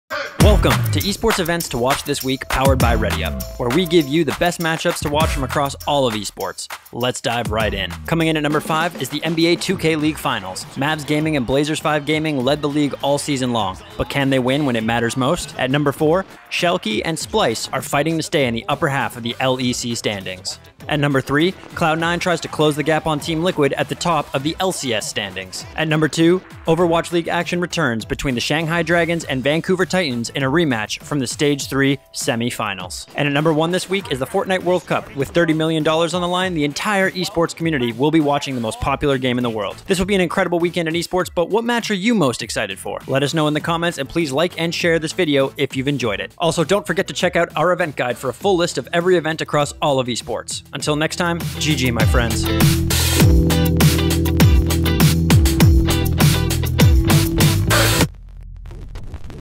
Esports Events to Watch July 22-28
Here are the top 5 events you don't want to miss this week! July 22-28th 2019.